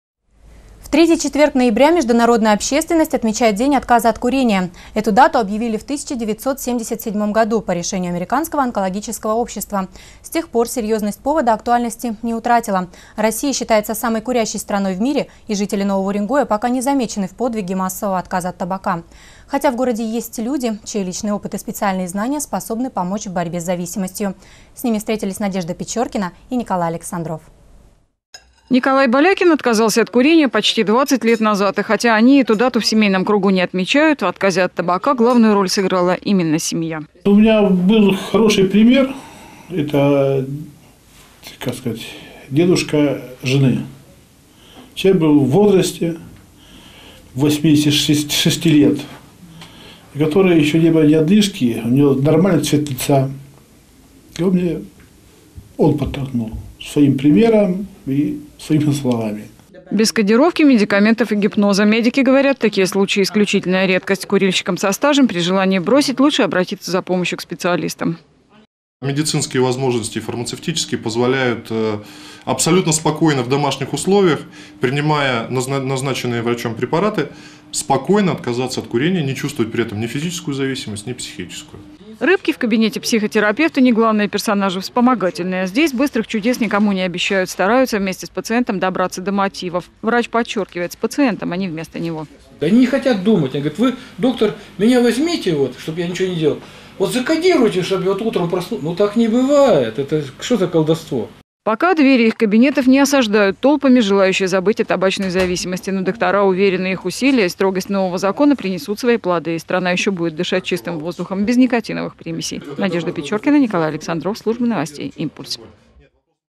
Сегодня отмечают международный день отказа от курения.
В третий четверг ноября международная общественность отмечает День отказа от курения. Эту дату объявили в 1977 году по решению Американского онкологического общества. С тех пор серьёзность повода актуальности не утратила. Россия считается самой курящей страной в мире, и жители Нового Уренгоя пока не замечены в подвиге массового отказа от табака. Хотя в городе есть люди, чей личный опыт и специальные знания способны помочь в борьбе с зависимостью.